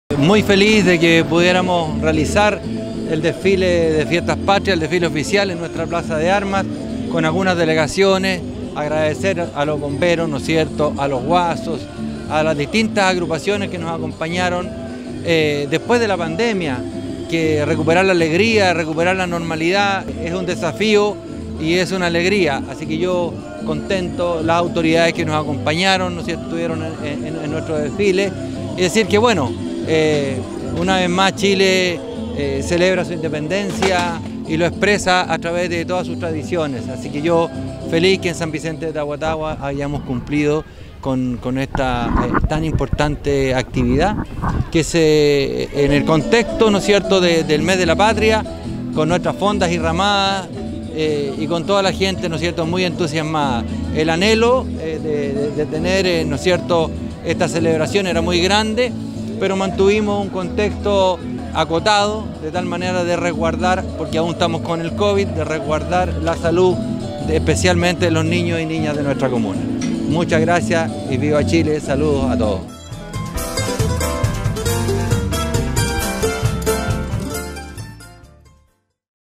FIESTAS PATRIAS 2022
Luego de casi dos años sin celebrar 18 de septiembre la Independencia de Chile, esta mañana nuestra plaza de armas fue nuevamente el escenario del desfiles de Fiestas Patrias.

Finalizado el acto, el Alcalde Jamie González Ramírez , agradeció a quienes participaron de esta tradicional actividad.